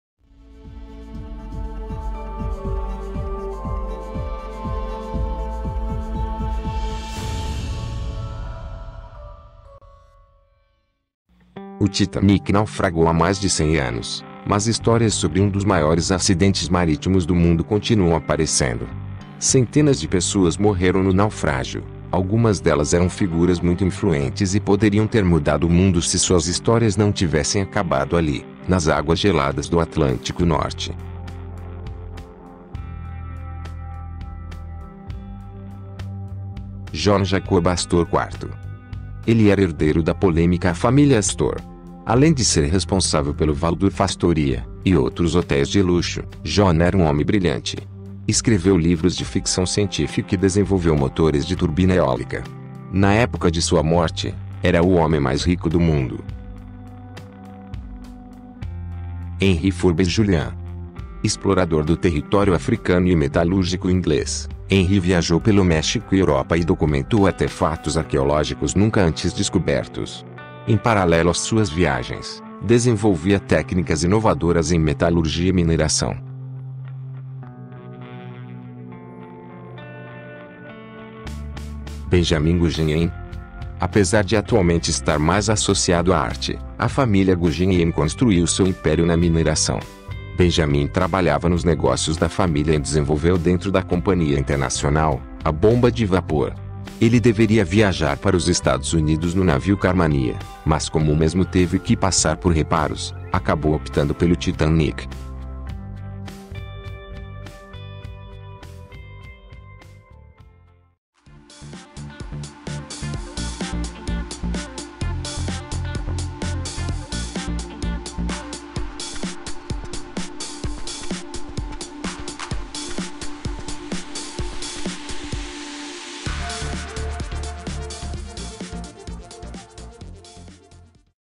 Se não tivessem morrido no Titanic, esses homens poderiam ter feito coisas Incríveis
O Titanic naufragou há mais de 100 anos, mas histórias sobre um dos maiores acidentes marítimos do mundo continuam aparecendo. Centenas de pessoas morreram no naufrágio; algumas delas eram figuras muito influentes e poderiam ter mudado o mundo se suas histórias não tivessem acabado ali, nas águas geladas do Atlântico Norte.